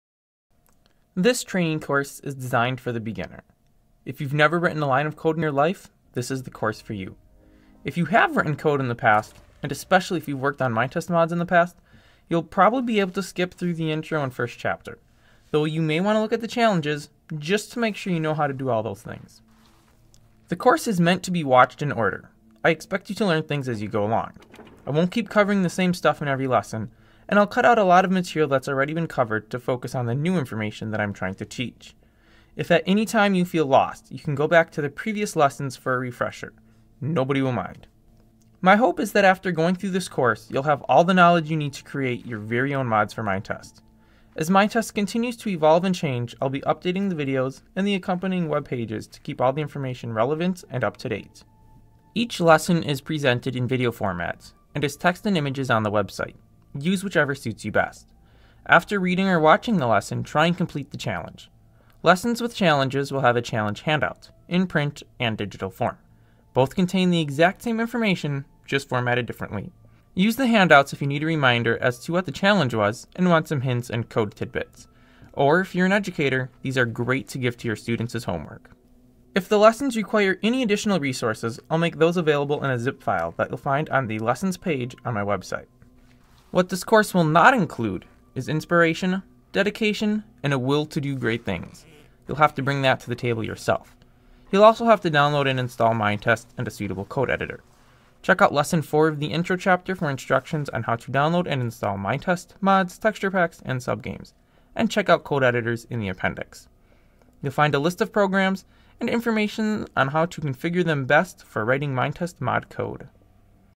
Course Overview | Minetest Modding Course
This course is designed with the beginner in mind, but even intermediate modders should be able to learn something.